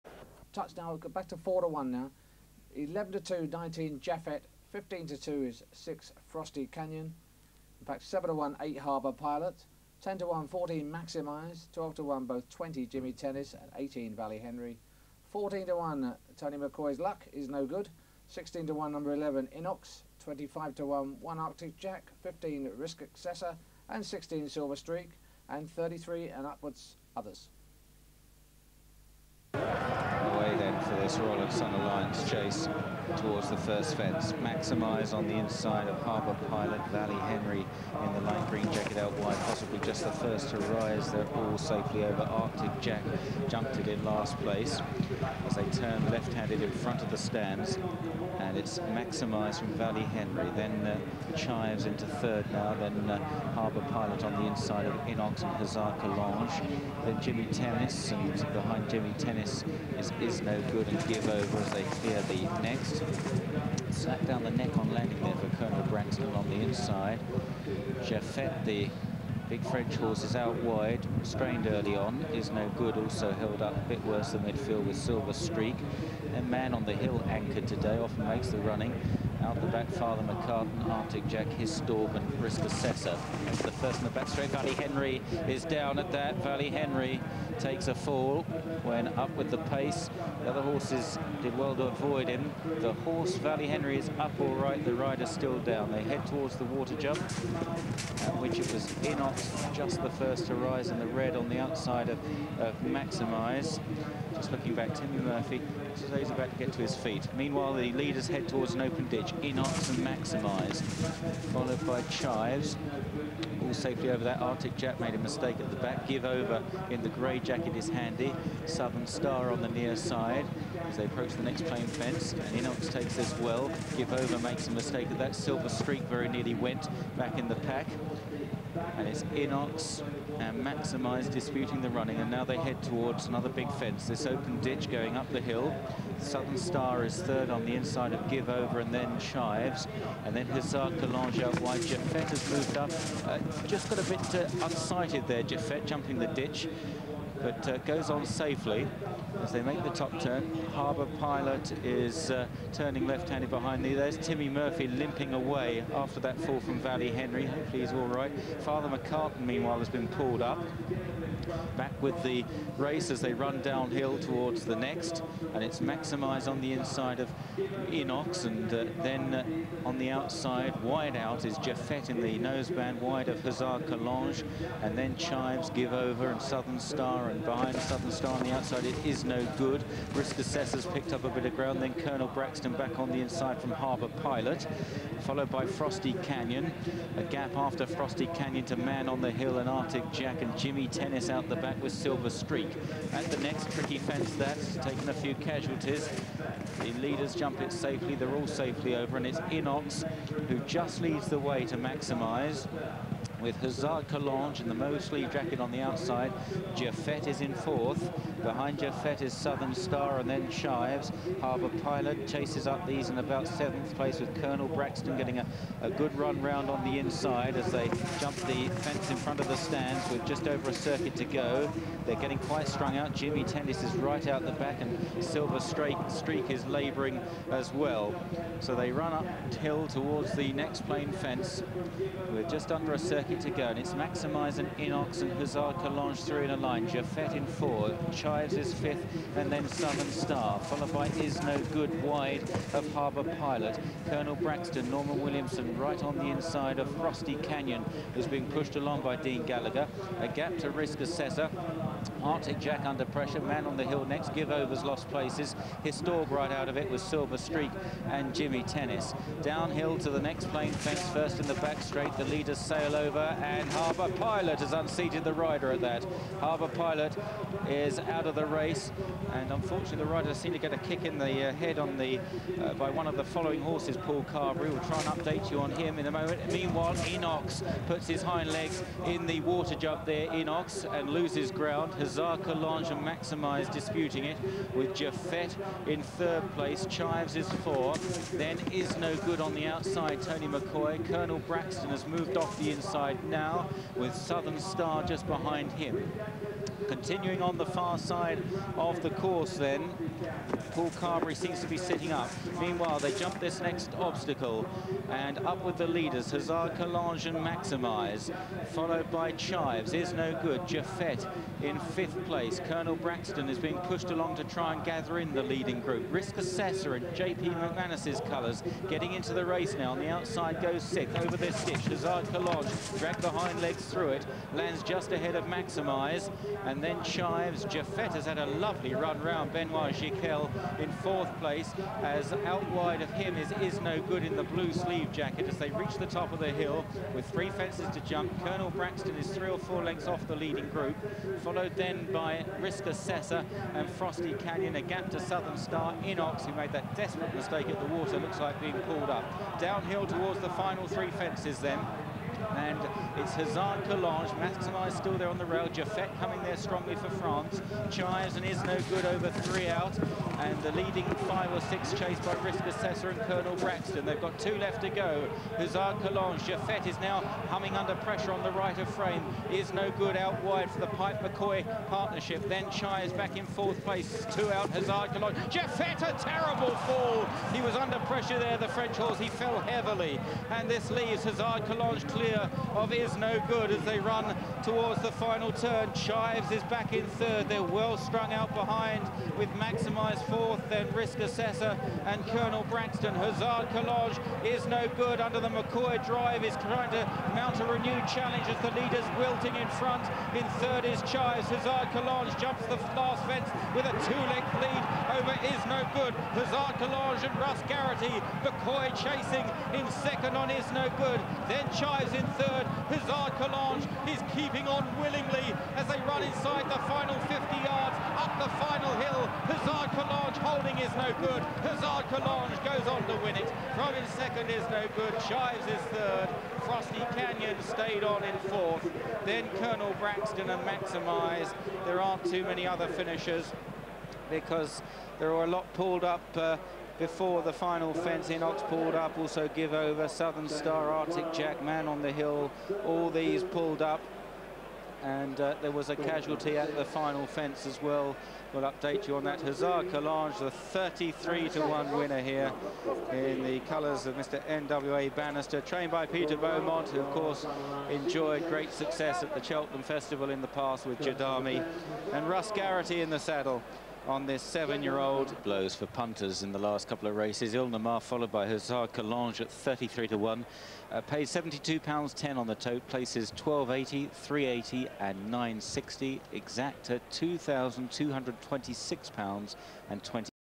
2002 Royal & Sunalliance Chase
Date: Wednesday 13th March 2002 |
Course: Cheltenham |
Distance: 3m 110y |
Going: Good to Soft |
Winning Time: 6m 35.65s |
Winner: Hussard Collonges (Russ Garritty) [P Beaumont] |
Runners: 19